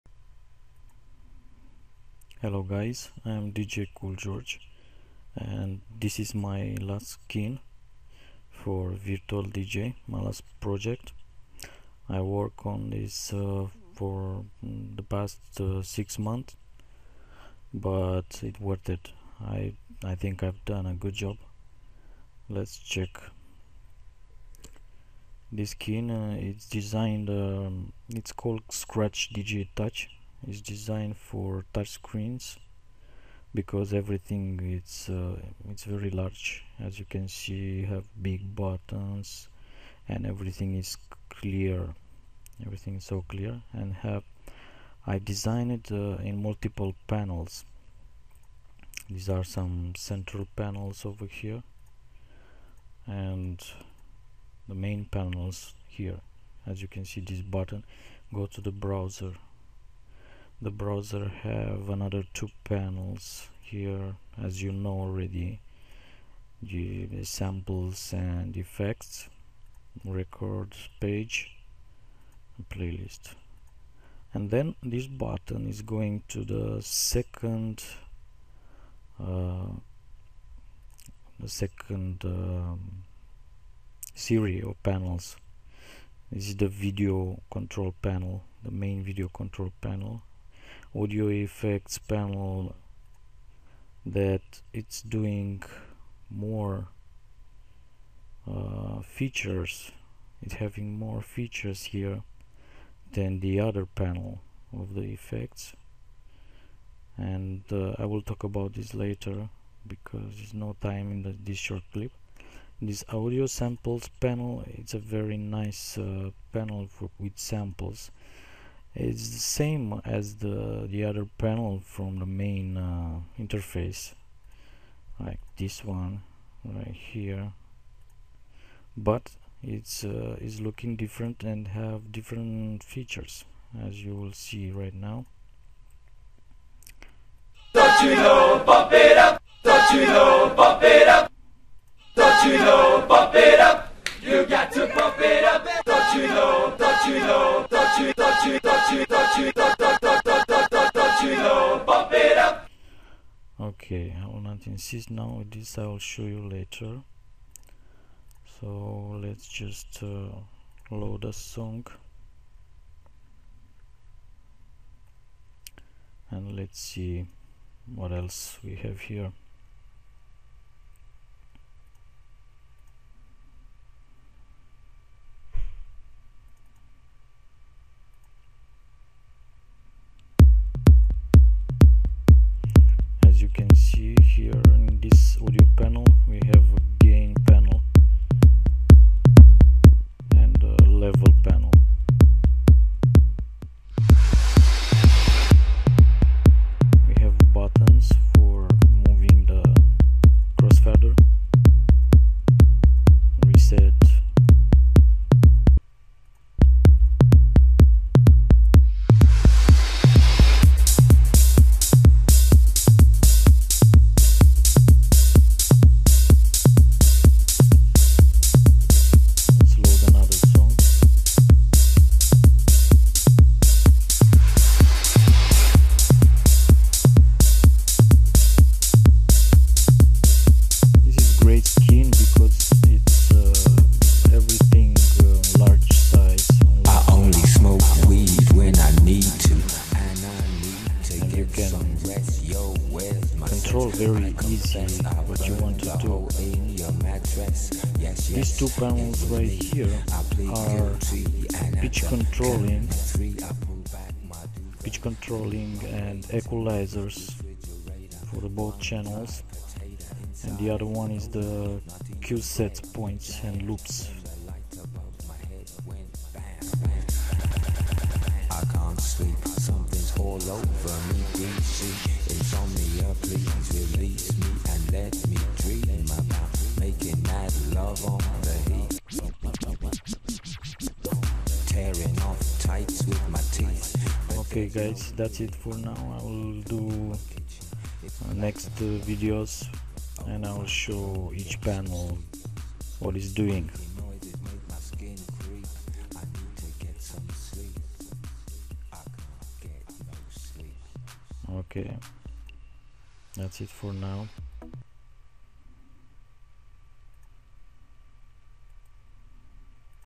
virtual dj skin
Skin for virtual DJ software,nice featured and great look...designed especially for touch screens(large buttons and panels).I will come up with more videos for this skin...If you want  this skin go on the link below...